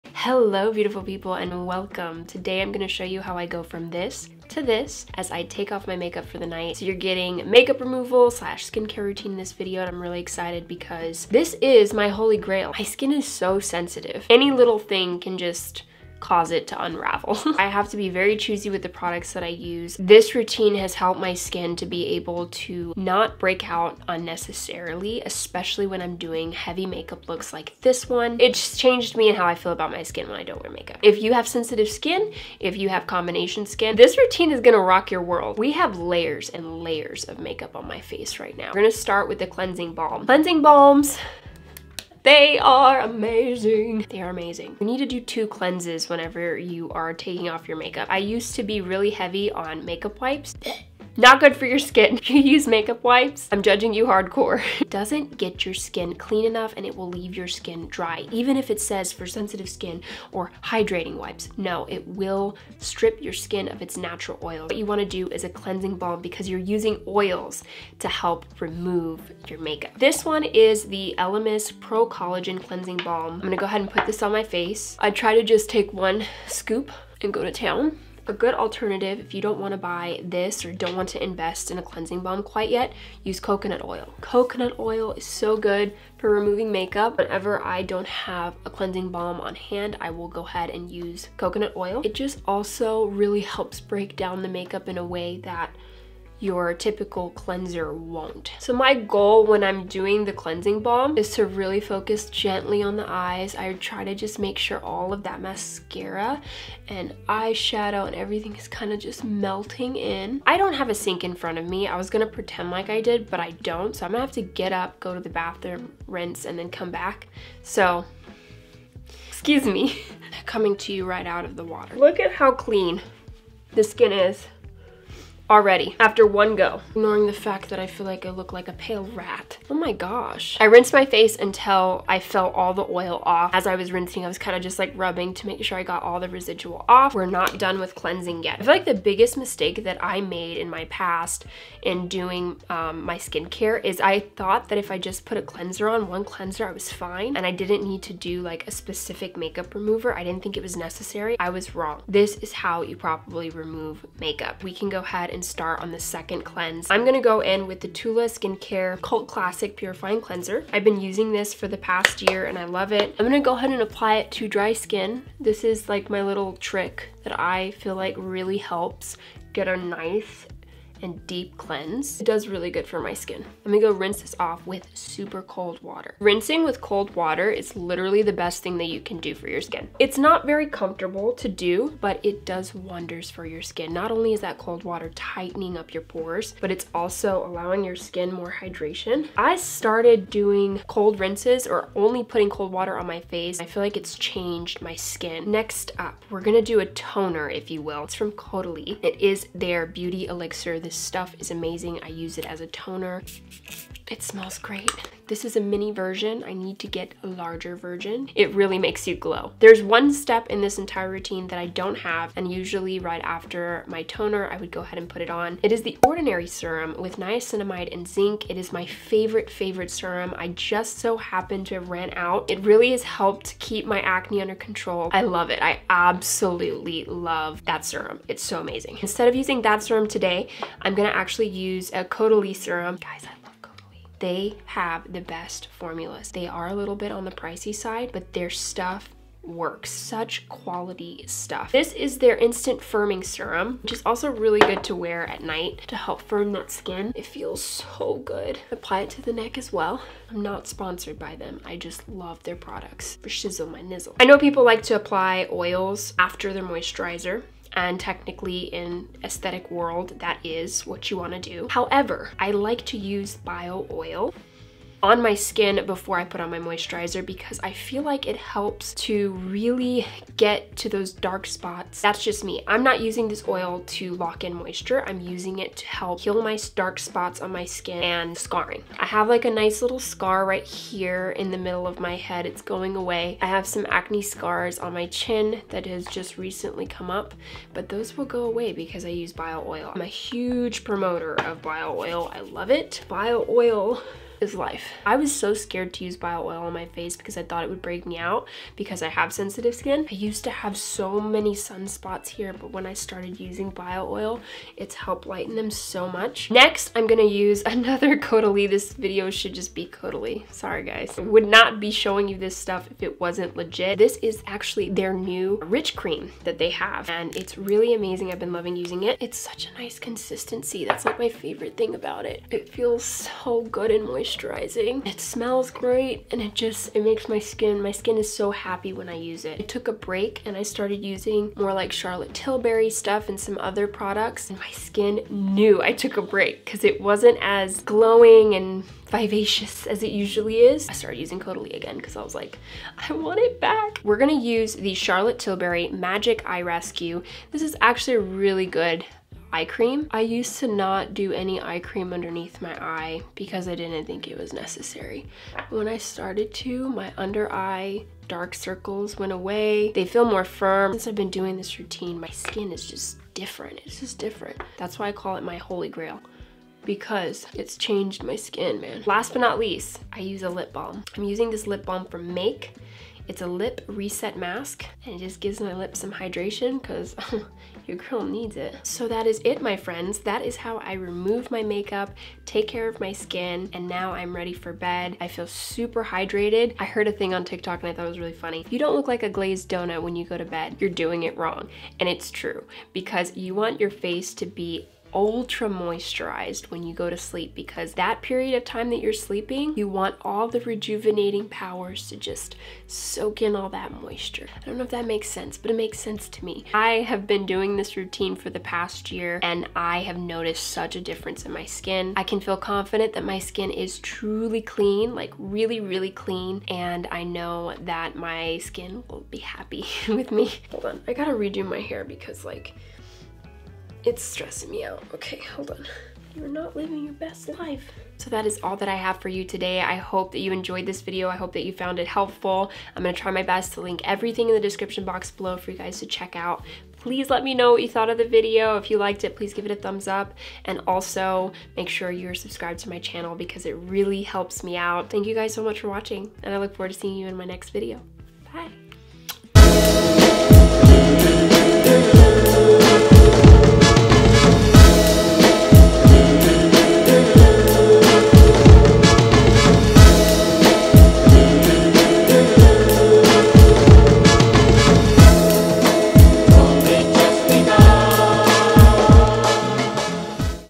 everyday skincare for sensitive skin
This is my go to easy makeup removal and skincare routine for the most healthy, hydrated and glowing complexion. I hope you enjoy:)

CATCH UP WITH ME DAILY:)